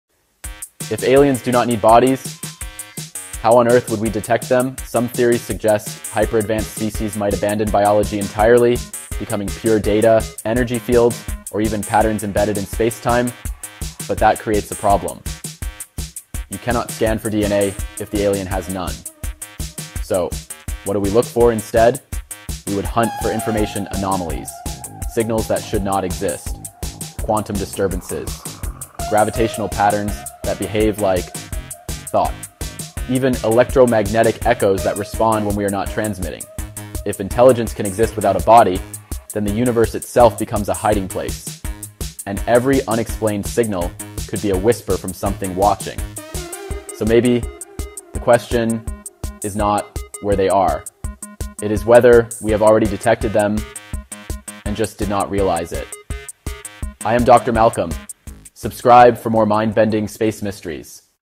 If Aliens Don’t Need Bodies, How Would We Detect Them?
What if aliens don’t need bodies at all? 🤔
In this mind-bending short, Doctor Malcolm explores the possibility of hyper-advanced beings existing as pure data, energy, or patterns woven directly into spacetime. If they’ve abandoned biology entirely… how would we ever detect them?

From tracking information anomalies and quantum disturbances to decoding gravitational patterns that behave like thought, the search for extraterrestrial intelligence might be far stranger and far harder than we ever imagined.
Could every unexplained signal be a quiet whisper from something watching?